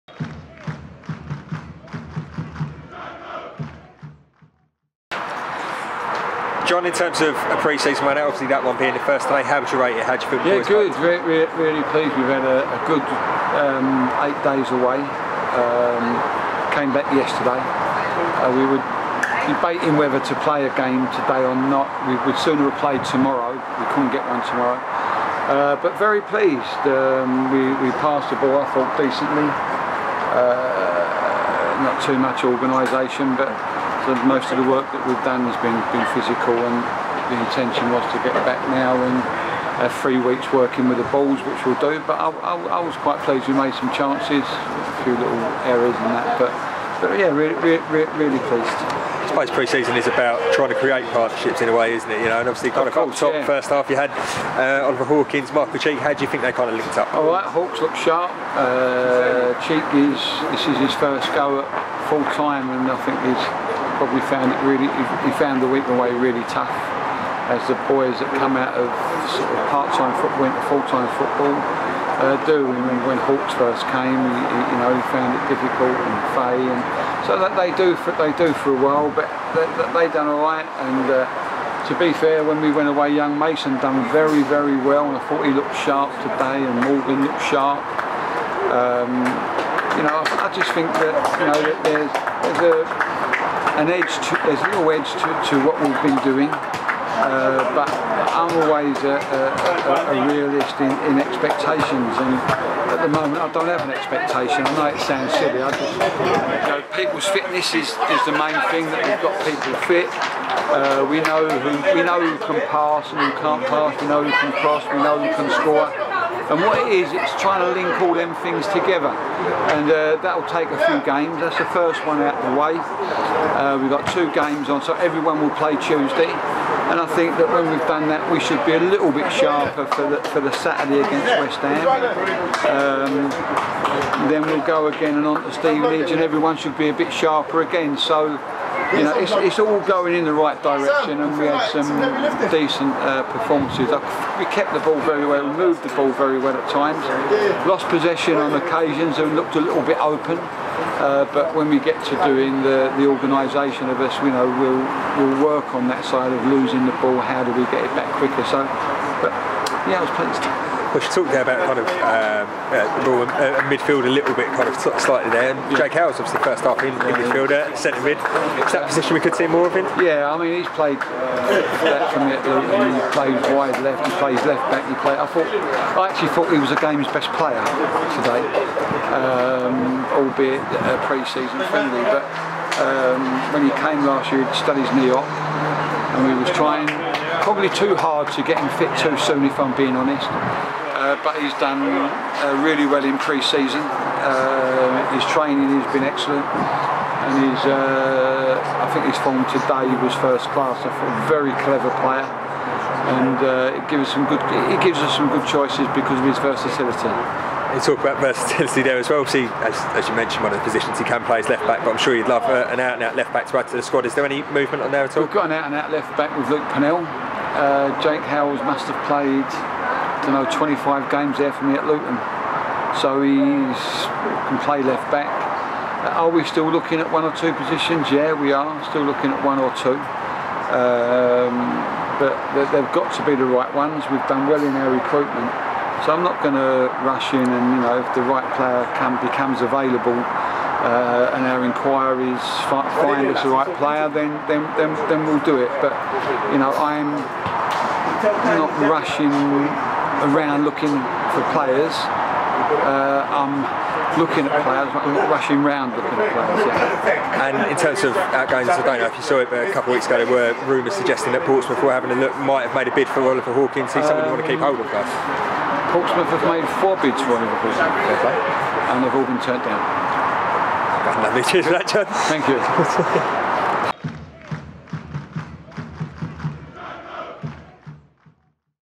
John Still Post Bedford Town
The Daggers media team spoke to Manager John Still after Saturday’s pre-season fixture against Bedford Town.